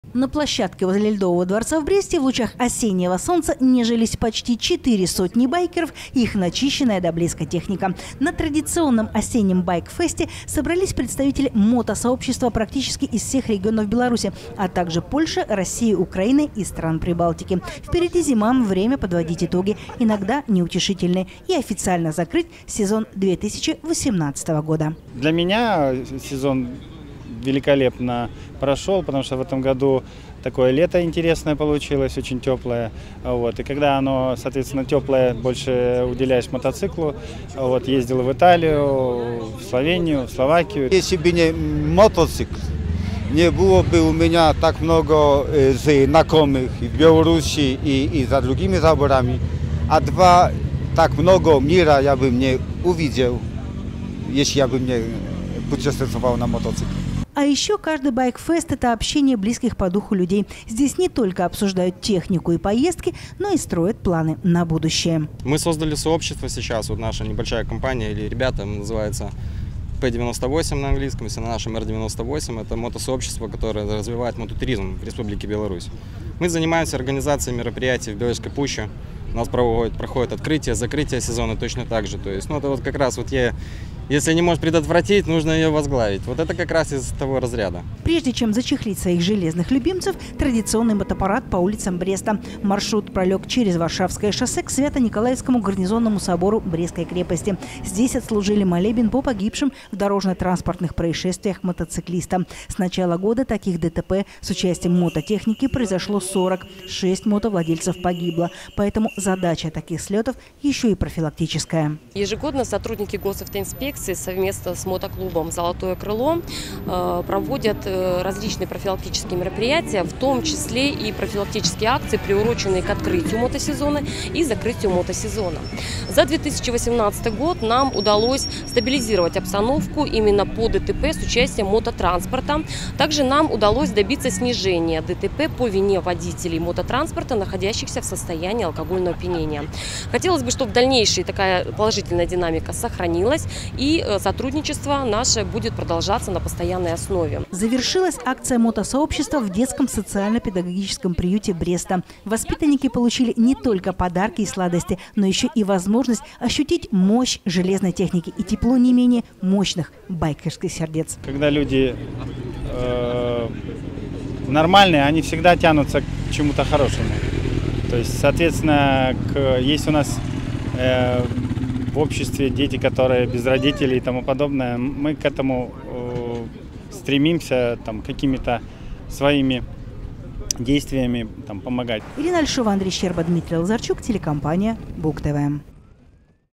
2018-10-09 г. Брест. Закрытие мотосезона. Новости на Буг-ТВ.
Первая неделя октября в Бресте отмечена интересным событием в жизни мотосообщества: поклонники и любители двухколесных железных коней официально завершили сезон. Традиционно точкой сбора участников осеннего байк-фестиваля, который проходит под патронажем брестского мотоклуба «Золотые крылья», стала площадка возле Ледового дворца. Говорили о печальной статистике, которая в этом году, к счастью, не дала положительной динамики, вспоминали сотни километров пройденных дорог и совместные путешествия, строили планы на сезон будущий.